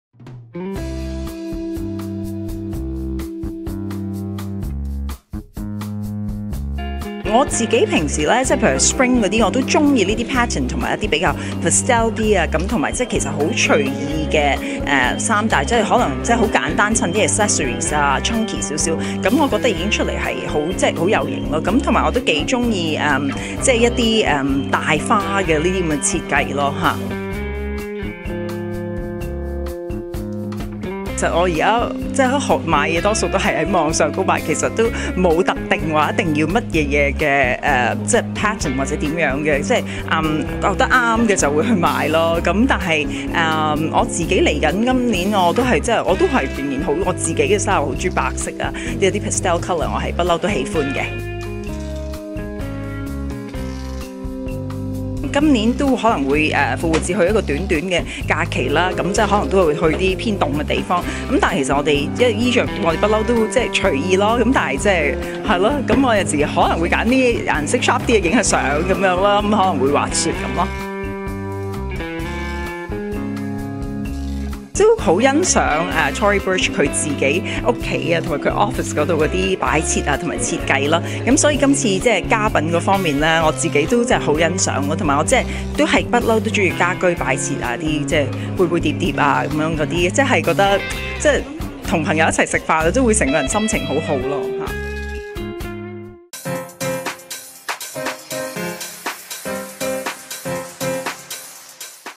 李嘉欣現身Tory Burch 分享襯衫tips
早前Tory Burch慶祝新店開幕，請來久未露面的李嘉欣於新店主持揭幕儀式，Michelle更分享了她的襯衫tips，但除咗買衫之外佢都好鐘意買家品，一齊黎睇下靚媽嘅選擇啦﹗